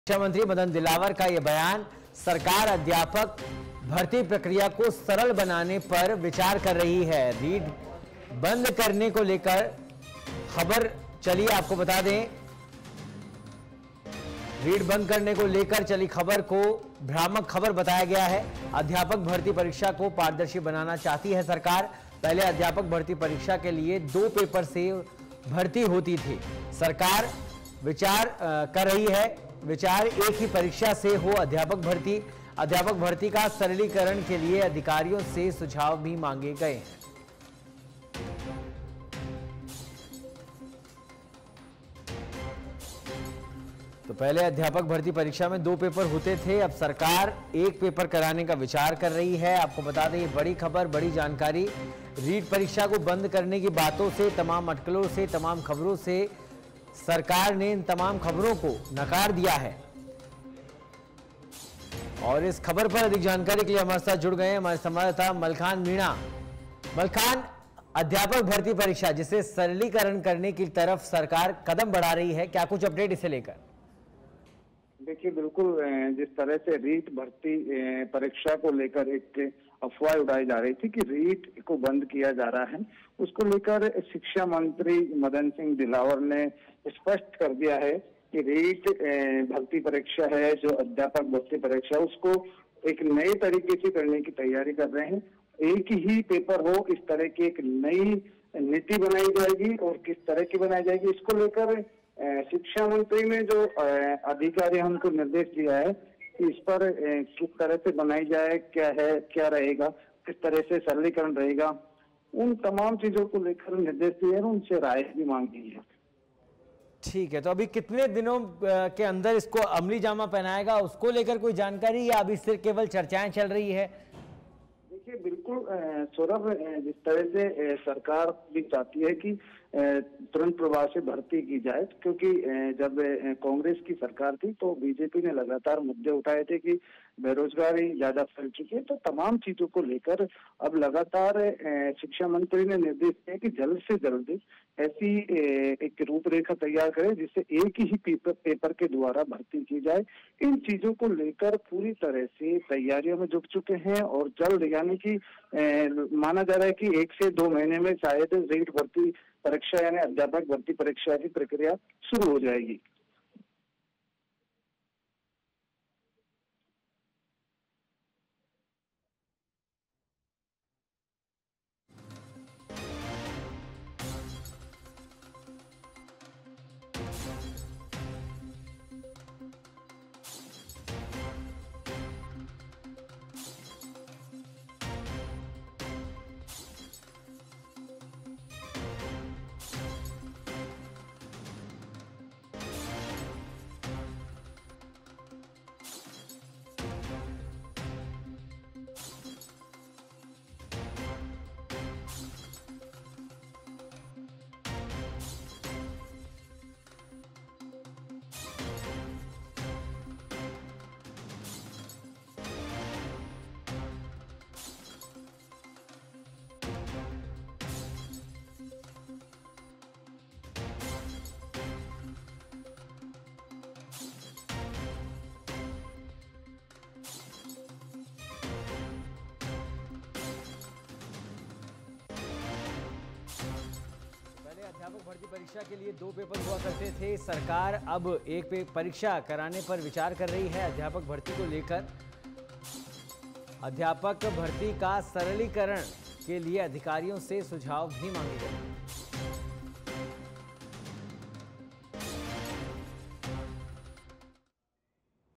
शिक्षा मंत्री Madan Dilawar रीट बंद करने को लेकर चली खबर को बताया भ्रामक खबर | Jaipur News
About Video: शिक्षा मंत्री Madan Dilawar रीट बंद करने को लेकर चली खबर को बताया भ्रामक खबर | Jaipur News